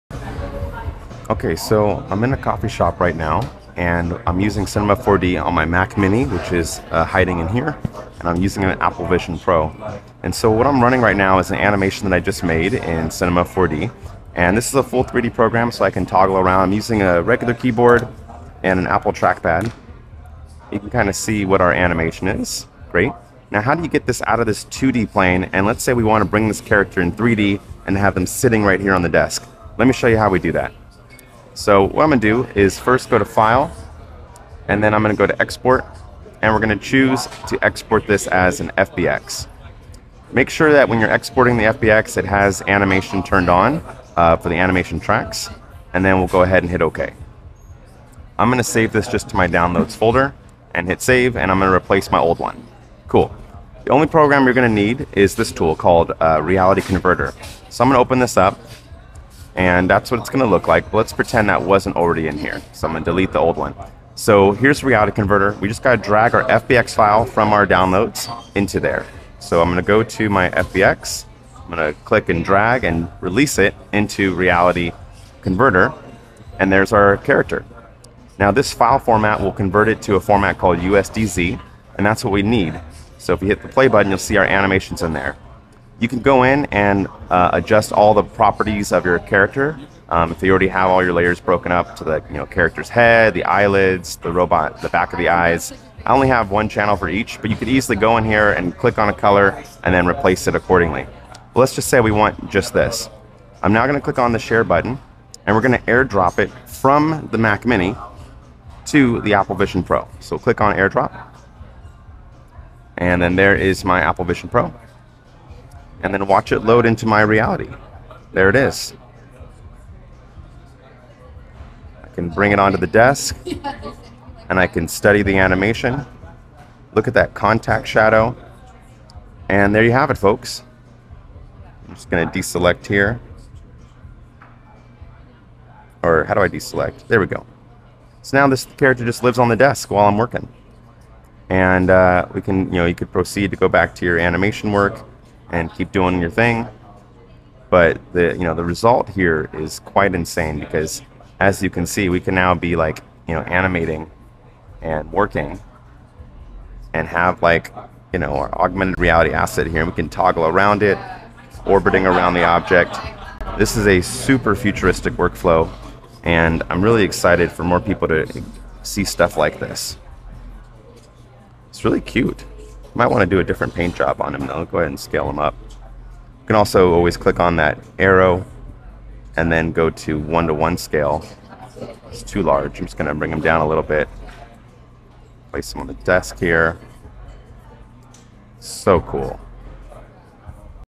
Mac Mini and Vision Pro in Public Cafe
✅ Mac Mini works with just VisionPro in public cafe! OMG I LOVE this workflow it's not just a gimmick it's practically and useful espicaly ppl in animation VFX and XR spaces!